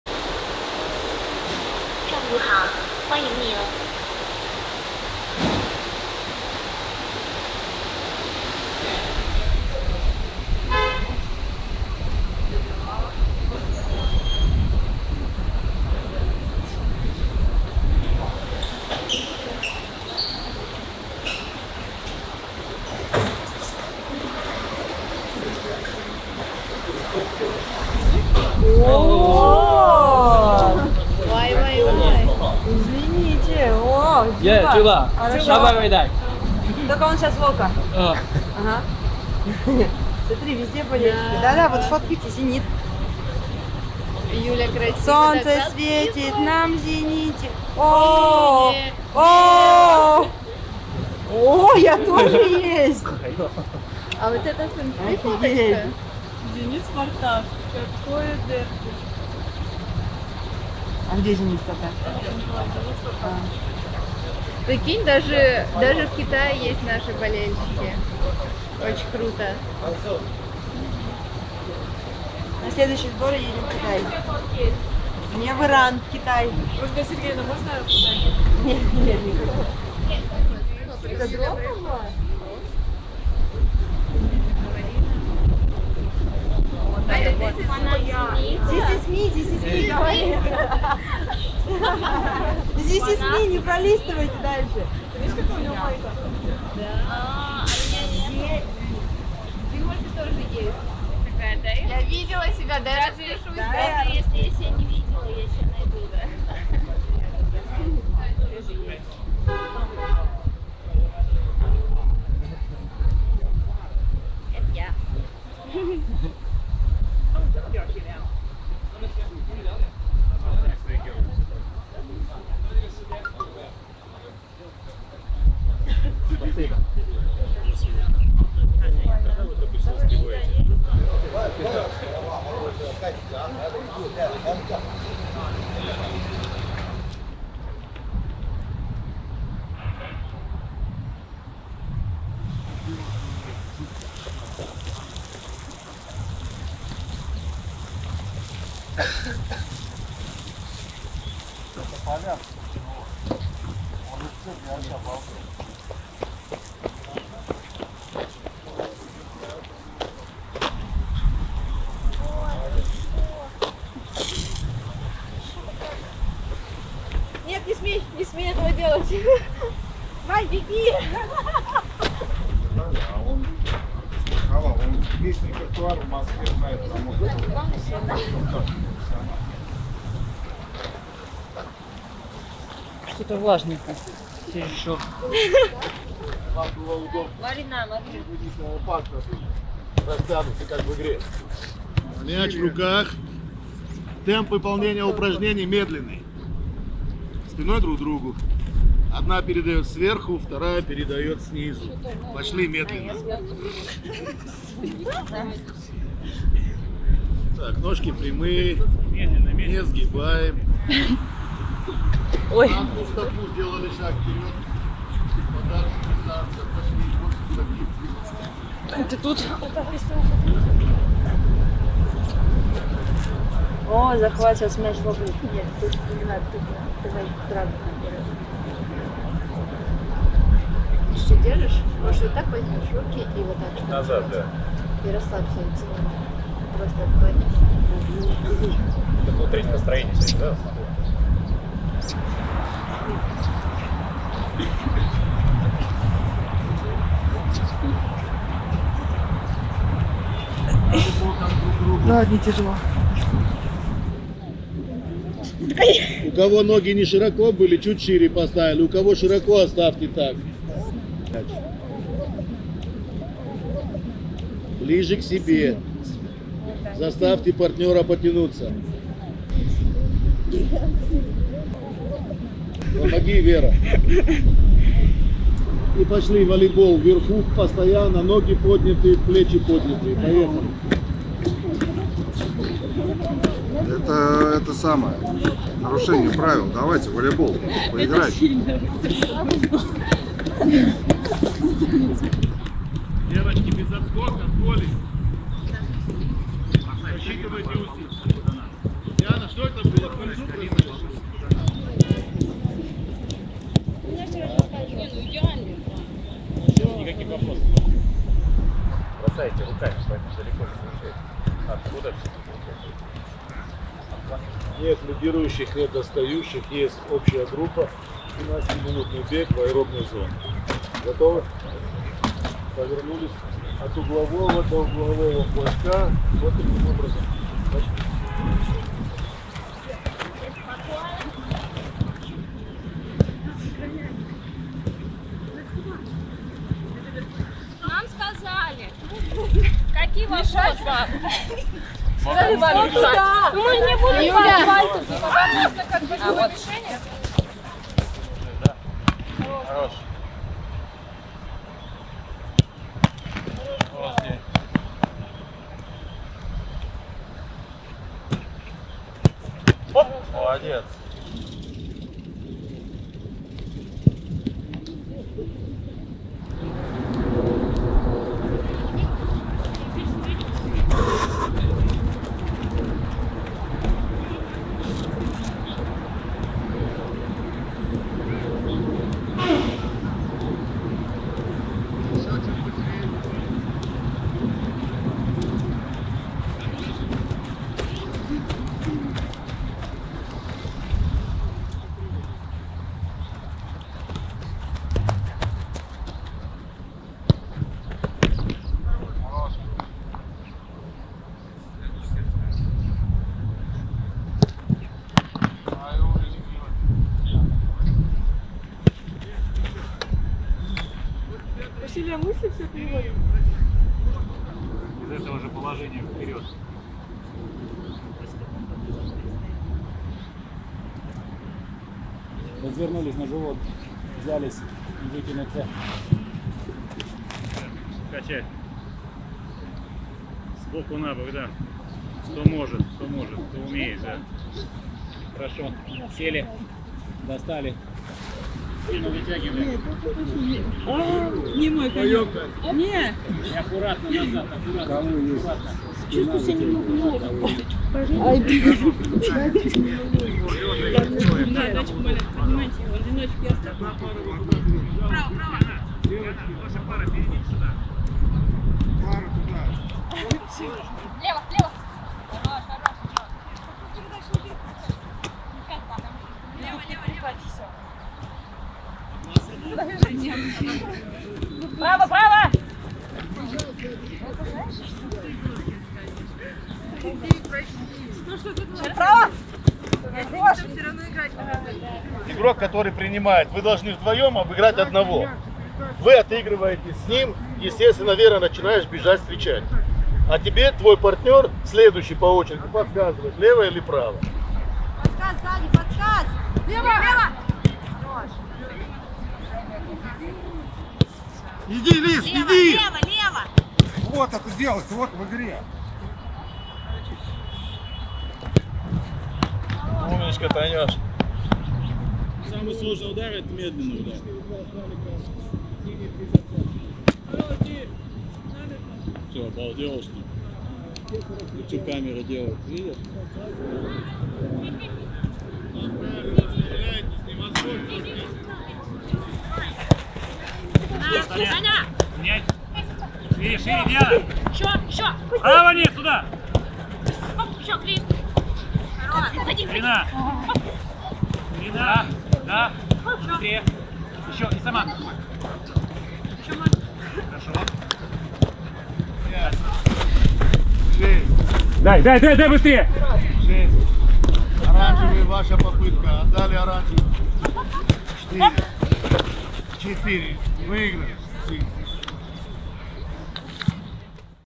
Сборы в Китае. День 7. Китайский фанат Дзюбы, мини-волейбол и восстановительная тренировка
Женская сборная России по футболу на учебно-тренировочном сборе в Китае провела восстановительную тренировку.

Перед автобусом наших девчат встретил робот и местный болельщик, а на стадионе их ждали интересные упражнения от тренерского штаба.

Подробности – в новом выпуске дневника сборов. Смотрите!